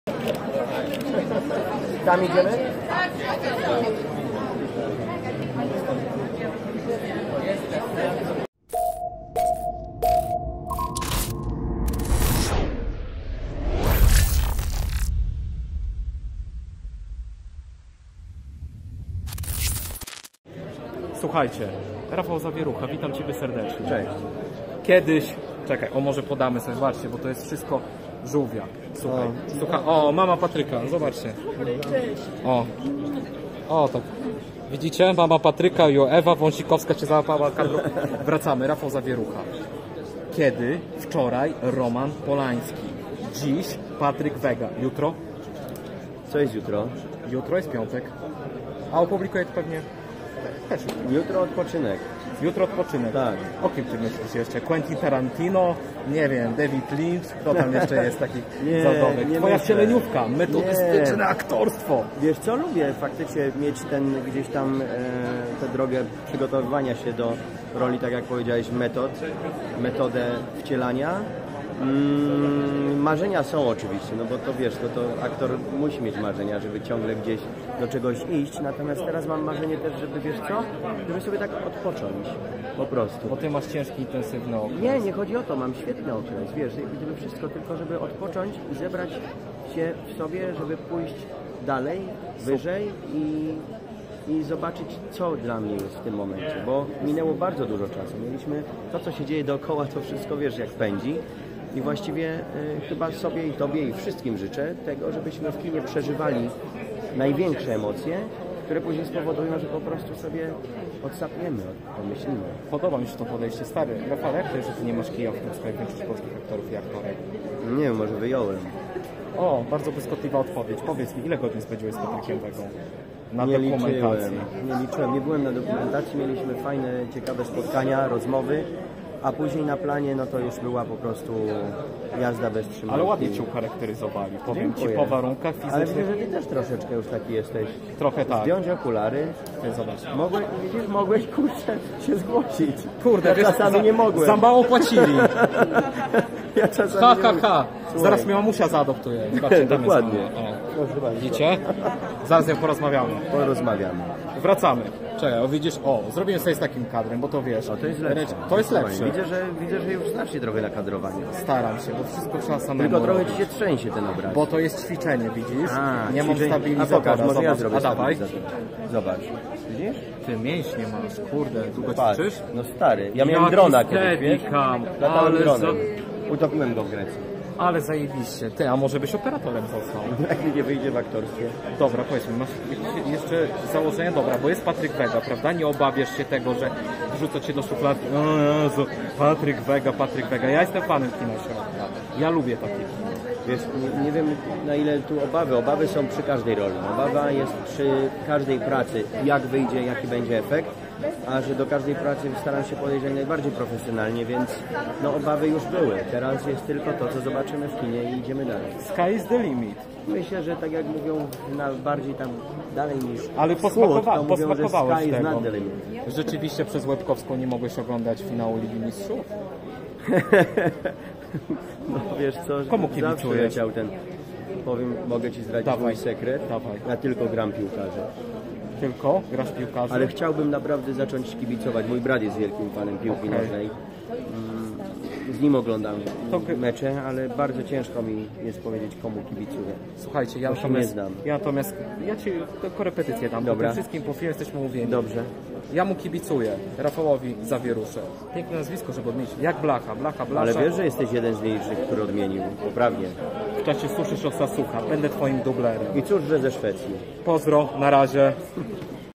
🎤[WYWIAD] RAFAŁ ZAWIERUCHA O ROLI PATRYKA VEGI W FILMIE PATRYKA VEGI🔥
🎞Zapraszam do obejrzenia  wywiadu z Rafałem Zawieruchą z oficjalnej premiery najnowszej produkcji Patryka Vegi Głównym tematem rozmowy jest  film "Niewidzialna wojna" w reżyserii autora "Pitbulla". Aktor zagrał oczywiście zagrał reżysera! Jak mu wyszło?

📣Obserwujcie moje profile w mediach społecznościowych.

Prowadzący: Bartosz Boruciak
Gość: Rafał Zawierucha